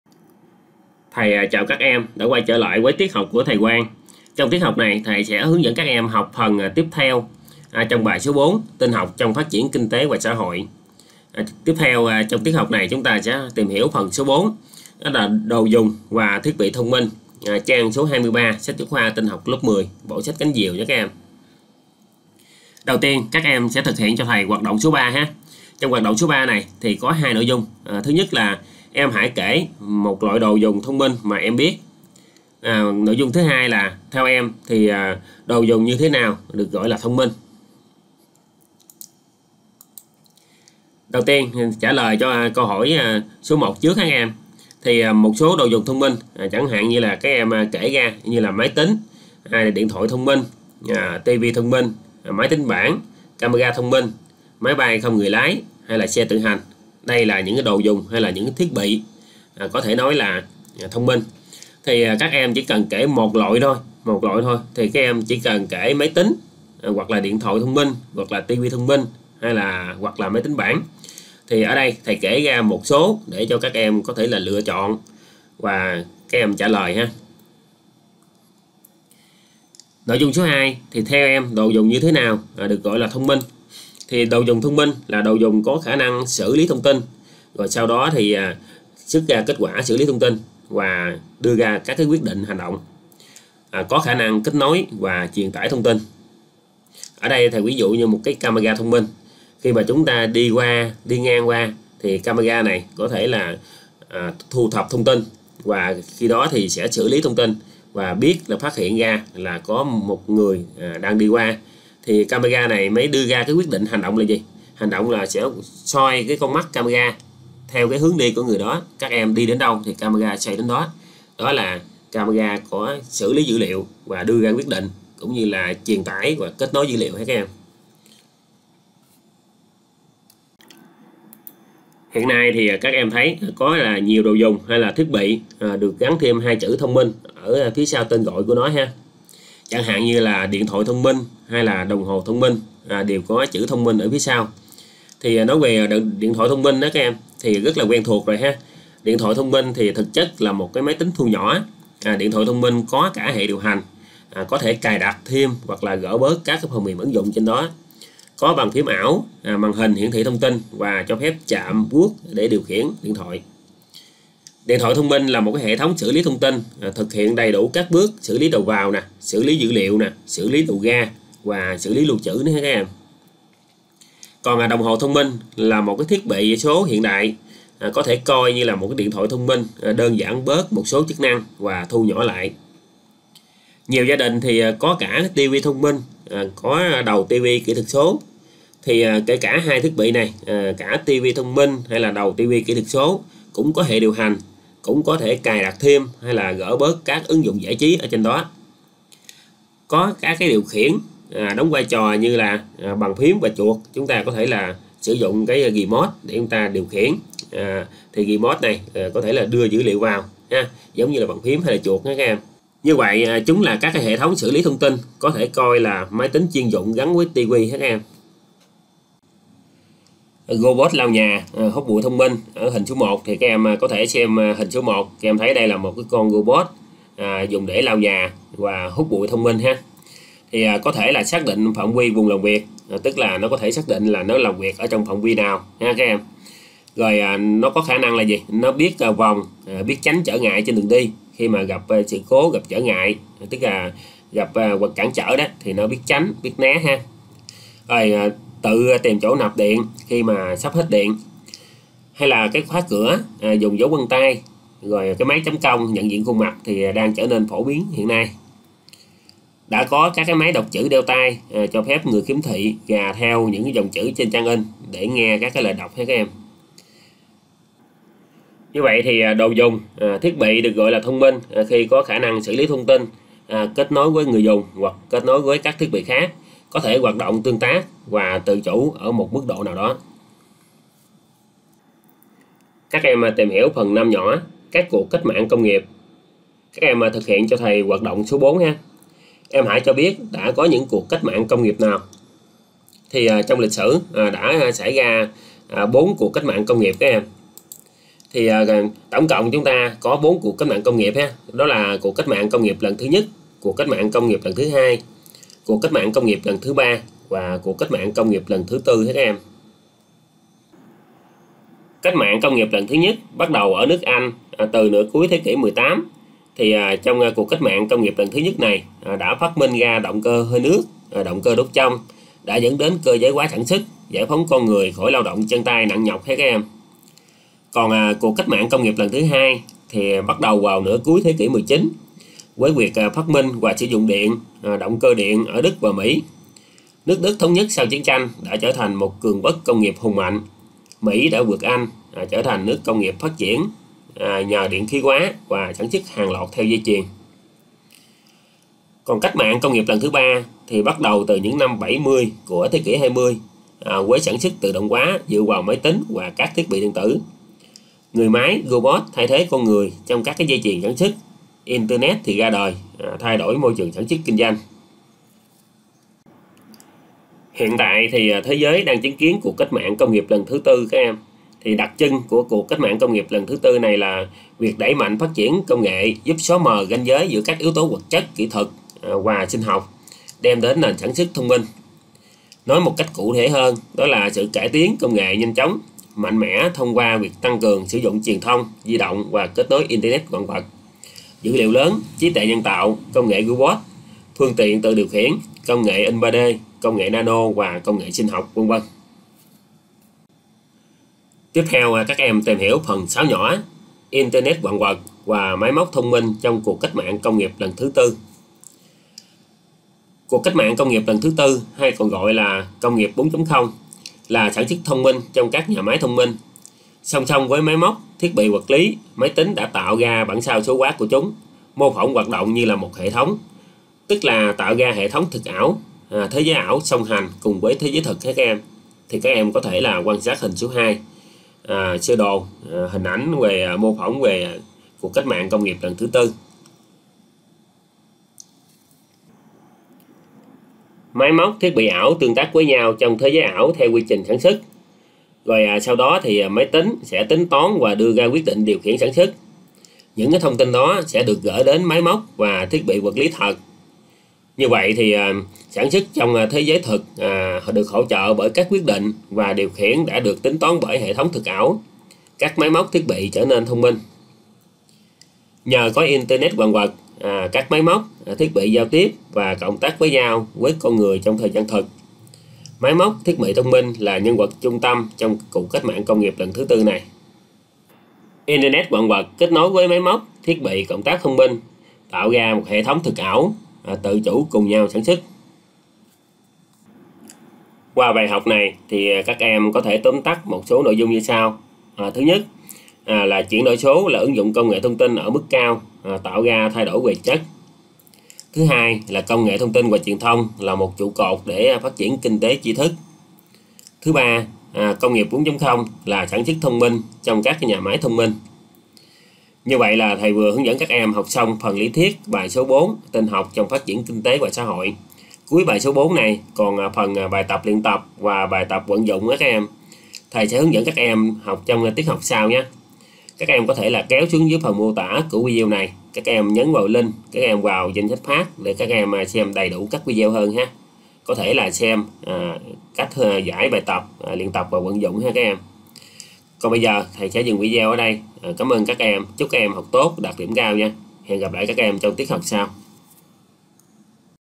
Bài 4 Chủ đề A Tin học 10 - Tin học trong phát triển Kinh tế Xã hội (tiếp theo) - Cánh diều
Bài giảng Tin học lớp 10, bài 4 Tin học trong phát triển Kinh tế - Xã hội (trang 20, tiếp theo), Chủ đề A Máy tính và xã hội tri thức (Tin học và xử lý thông tin), sách giáo khoa Cánh diều
Danh sách phát "Tin học 10 - Cánh diều", xem tất cả bài giảng tin học 10
Chúc các em học tập tốt và đạt điểm cao!